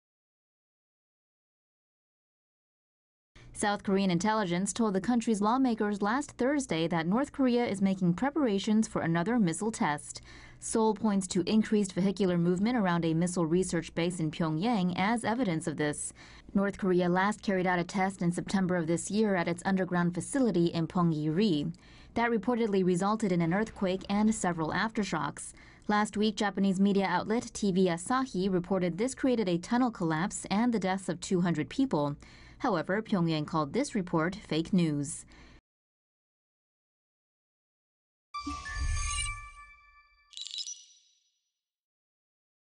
North Korea maybe readying for new missile test
RESTRICTIONS: NONE

North Korea maybe preparing for another missile test, South Korean intelligence told Seoul lawmakers last Thursday.

Seoul points to increased vehicular movement around a missile research base in Pyongyang as evidence of this, reported Reuters.

North Korea last carried out a test in September 2017 at its underground facility in Punggye-ri. That reportedly resulted in an earthquake and several aftershocks.

Last week Japanese media outlet TV Asahi reported this created a tunnel collapse and the deaths of 200 people, according to CNN. Pyongyang said this was fake news.

RUNDOWN SHOWS:
1. South Korea and North Korea on map
2. Depiction of vehicle movement inside North Korea
3. Depiction of North Korea's Punggye-ri test site
4. Depiction of explosion and earthquake in Punggye-ri.

VOICEOVER (in English):

"South Korean intelligence told the country's lawmakers last Thursday that North Korea is making preparations for another missile test."

"Seoul points to increased vehicular movement around a missile research base in Pyongyang as evidence of this."

"North Korea last carried out a test in September of this year at its underground facility in Punggye-ri."

"That reportedly resulted in an earthquake and several aftershocks."

"Last week Japanese media outlet TV Asahi reported this created a tunnel collapse and killed 200 people."

"However, Pyongyang called this report fake news."